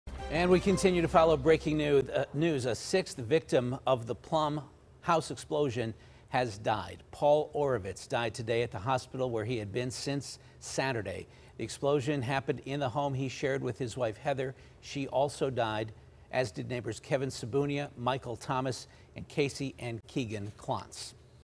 6th person dies after house explosion in Plum
Paul Oravitz died Wednesday, four days after the deadly blast in the Rustic Ridge neighborhood.